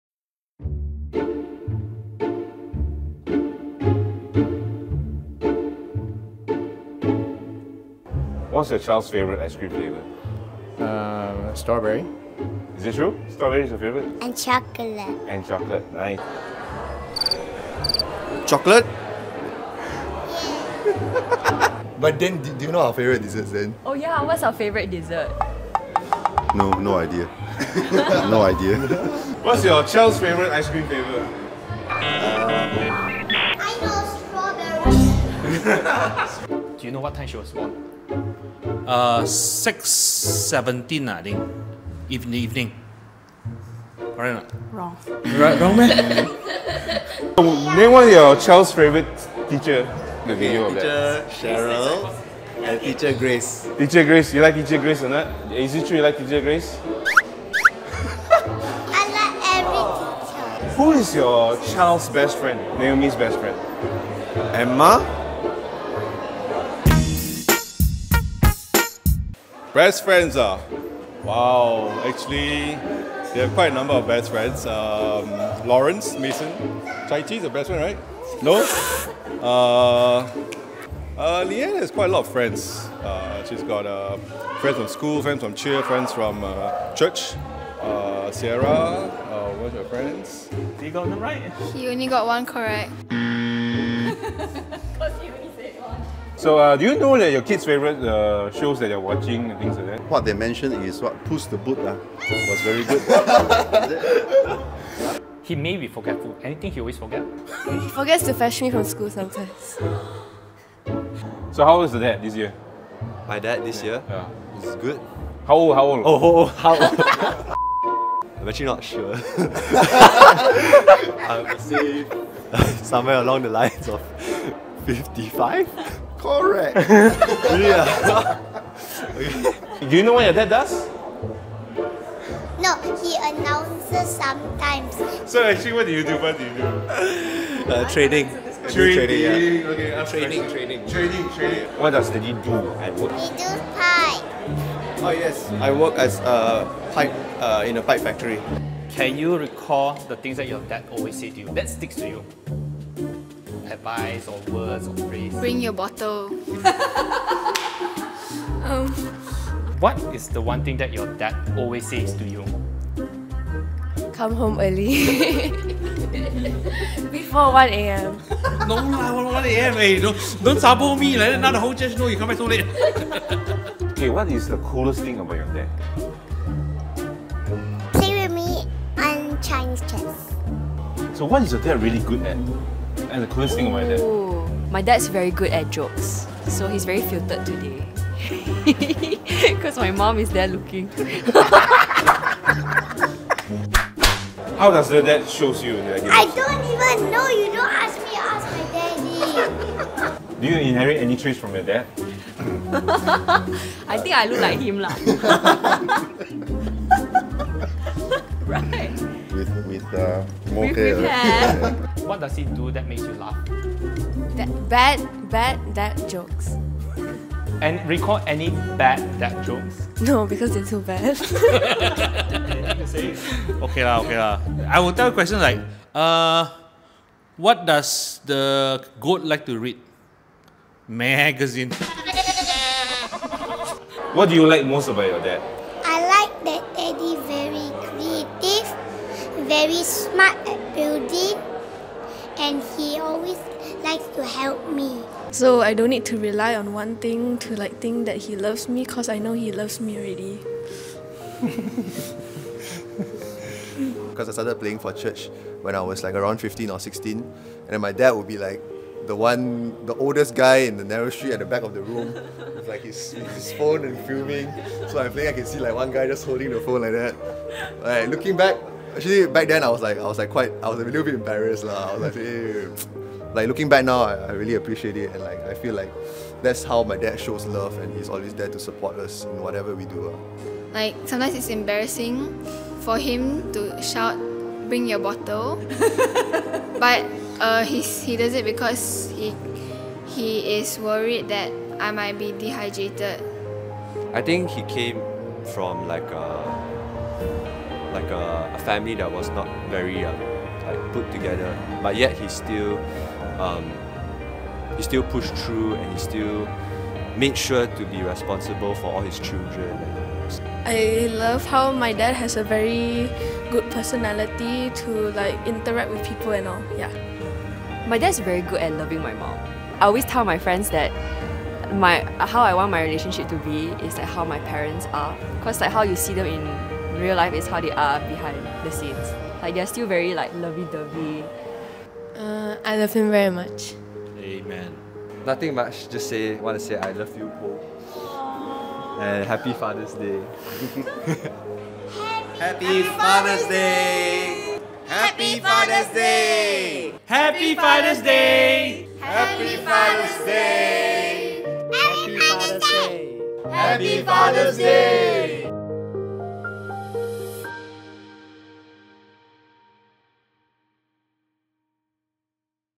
A Father's Day Video That Will Make You Laugh | How Well Do You Know Your Dad or Kid? | SIBKL Church
HAPPY FATHER'S DAY 2023!

Fathers are the pillar of any family. They may be stern, loving, supportive, funny (or are they), But whatever it is, they love their kids to bits and there are various ways a father expresses their love to them.

In this video, we did a light exploration of how different fathers within SIBKL know and understand their kids, and how their kids understand them.